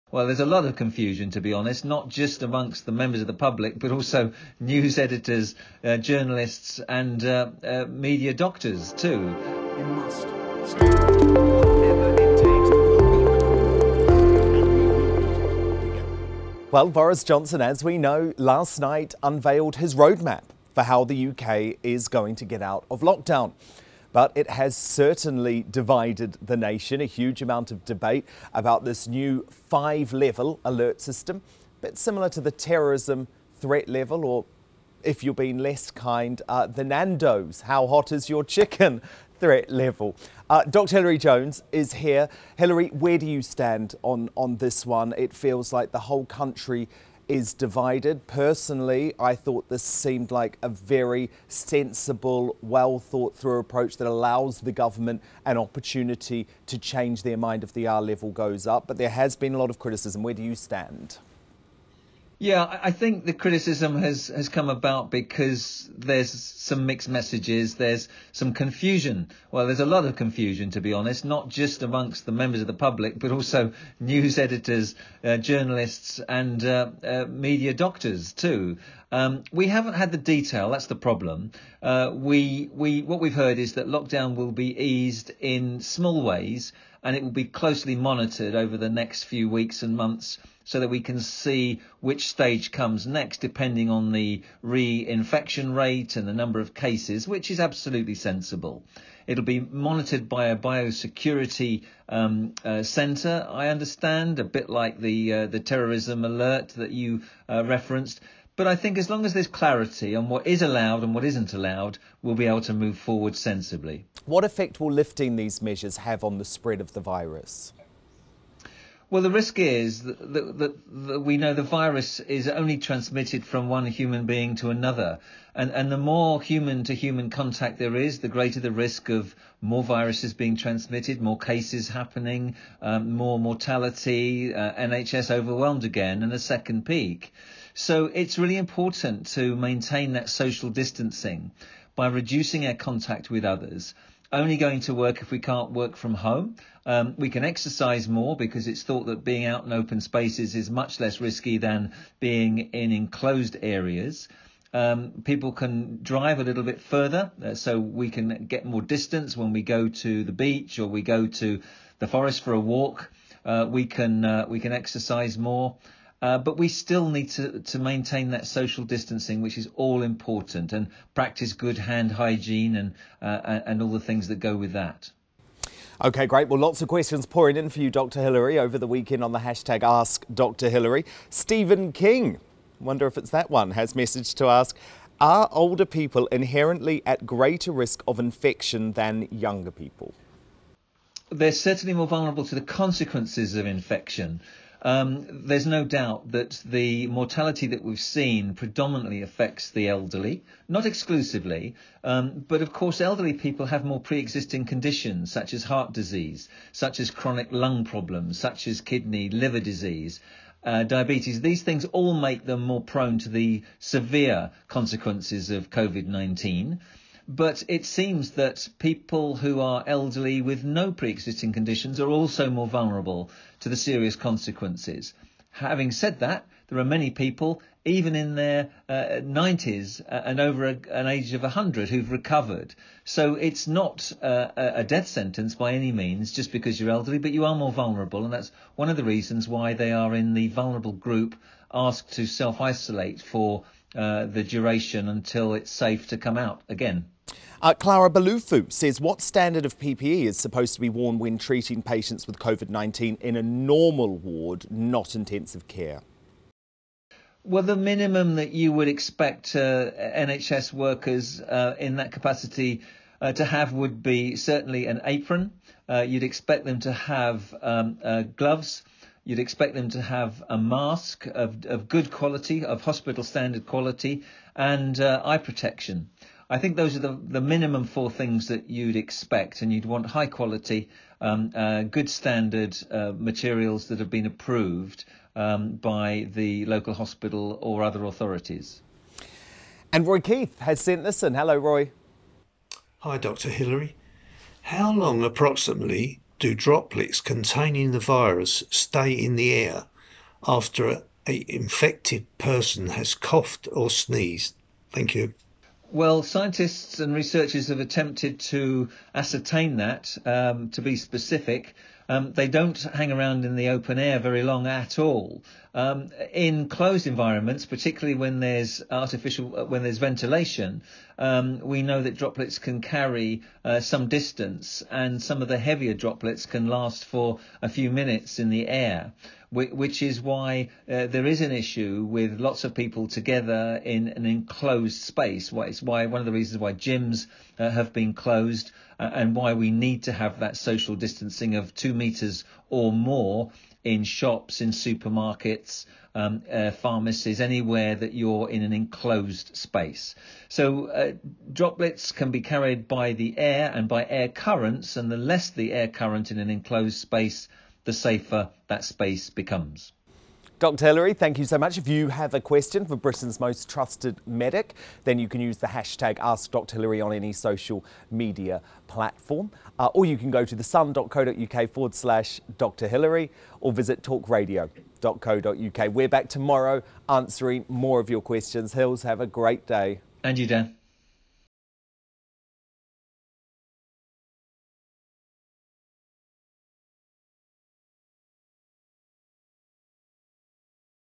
Coronavirus Explained: will Boris Johnson’s ‘roadmap’ to lift the UK’s COVID-19 lockdown work?
Prime Minister Boris Johnson has today outlined a detailed plan for the UK to exit the draconian and economically-crippling lockdown in place since March 23.

The 50-page document follows a speech to the nation aired last night in which the PM announced a five-tier virus threat alert system, told those who couldn’t work from home to return to their jobs and extended Brits’ permitted outdoor time to unlimited exercise and some public leisure activities.

But, will the government’s exhaustive plan to get Britain back up and running work?

Here to answer that and more Sun reader questions is Tv’s most-trusted medic Dr Hilary Jones.

In today’s episode of Ask Dr Hilary: Coronavirus Explained…

- Are older people inherently at greater risk of infection than younger people?

- What standard of PPE is supposed to be worn when treating patients with CVID-19 in a normal ward – not intensive care?

- How long, approximately, do droplets stay in the air after someone has coughed or sneezed with the virus.